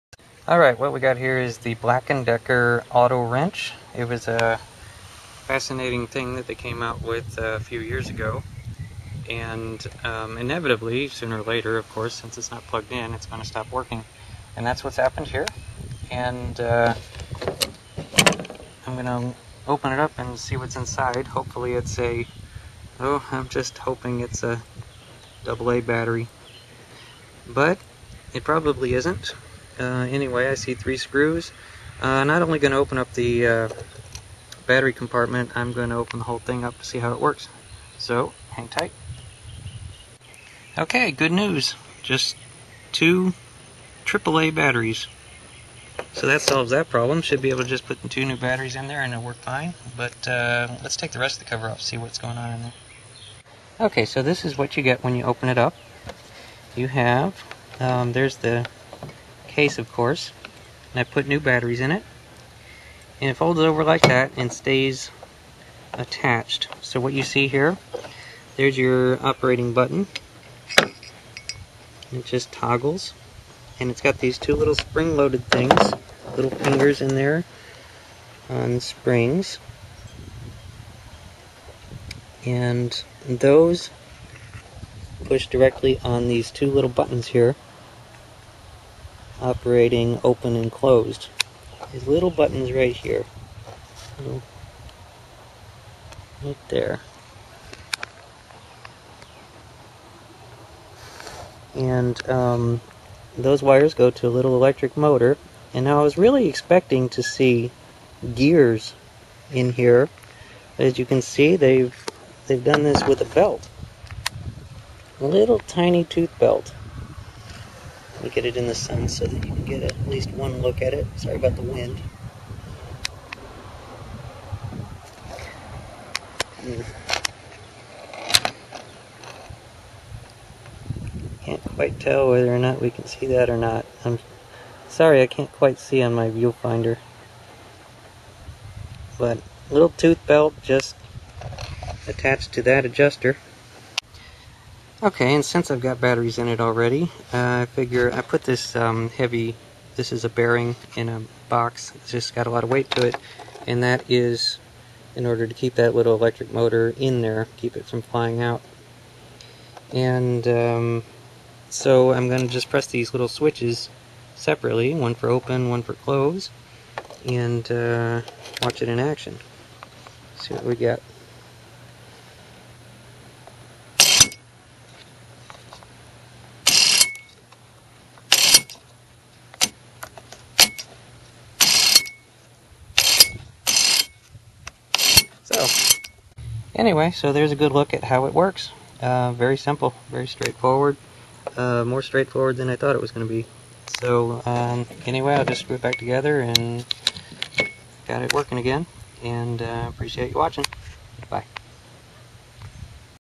Black & Decker Auto Wrench- What's Inside?
How does the Black & Decker Auto Wrench work? I opened it up to see what's inside. This wrench uses 2 AAA batteries.